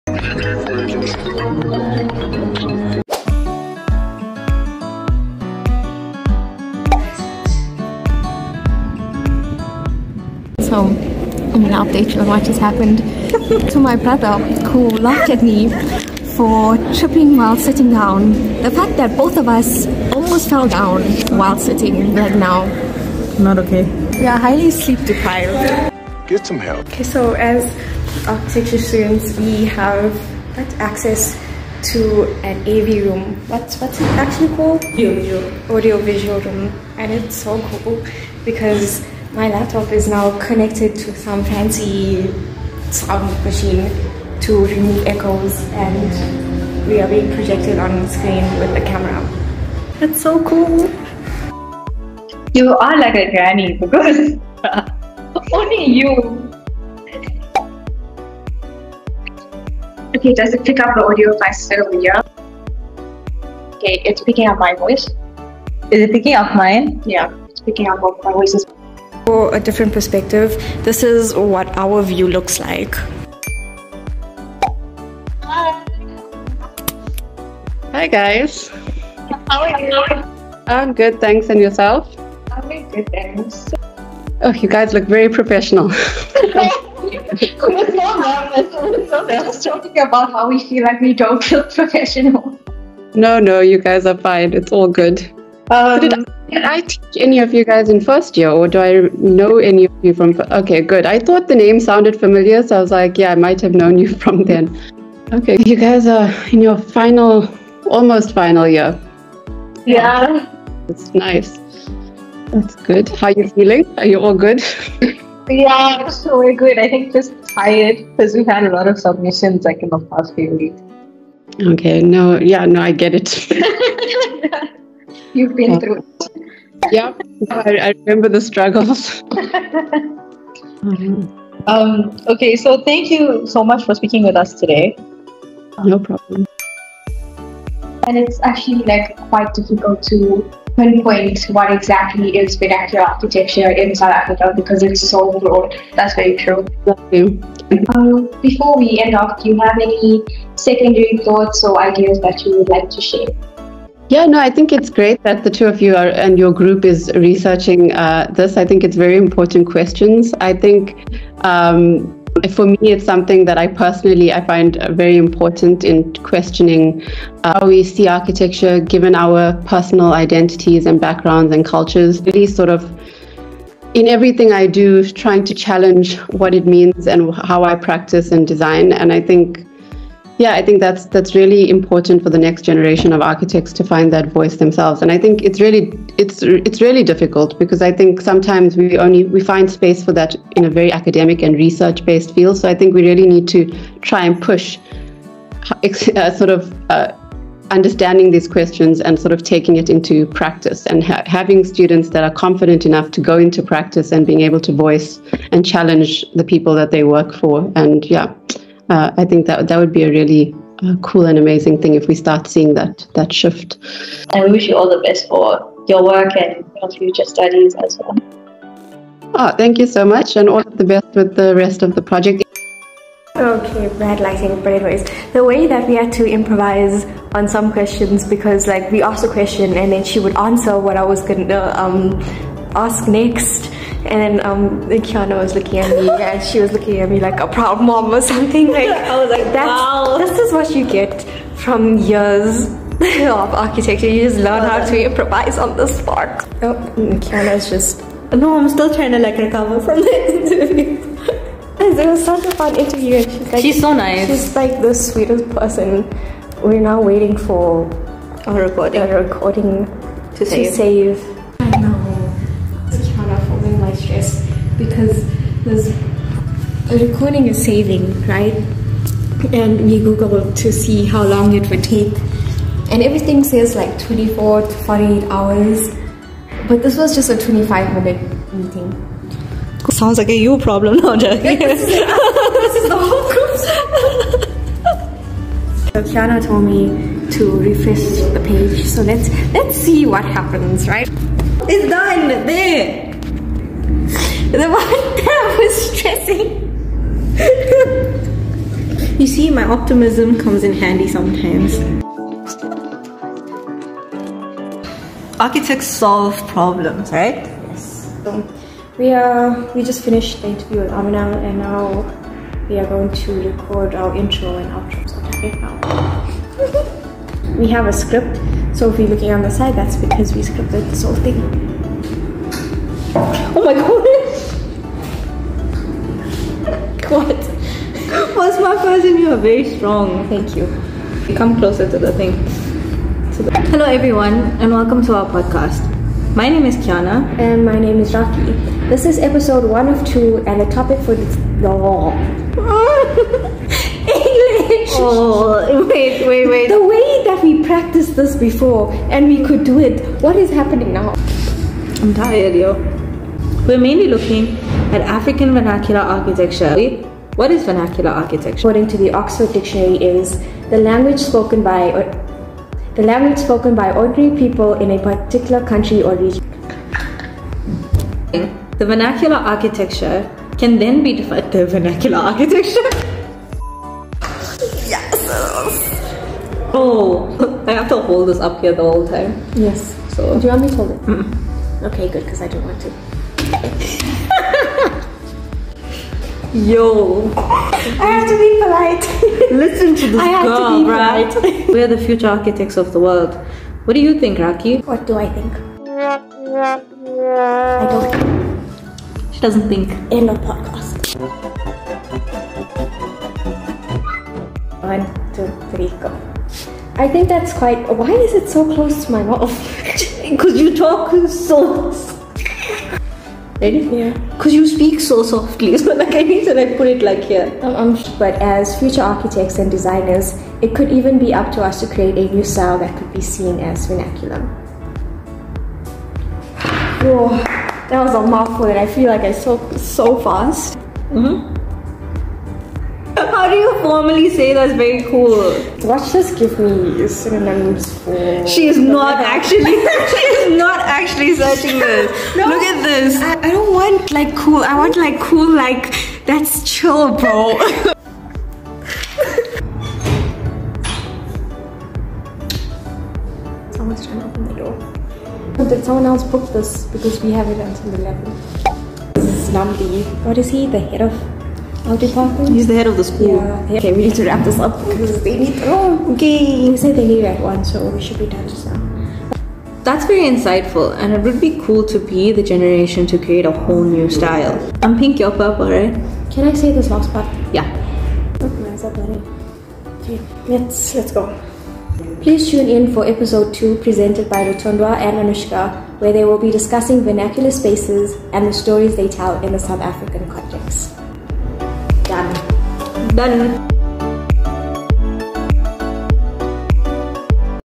BEHIND THE SCENES-podcast ft AMINA KASKAR| Day in the life| Architecture Student
The Friday following up on vlog 15.
Kiana and I interview Amina Kaskar for an interview - third year history project.I show parts that didn't make the podcast and the behind the scenes of recording our intro and outro as well as the set-up for the interview.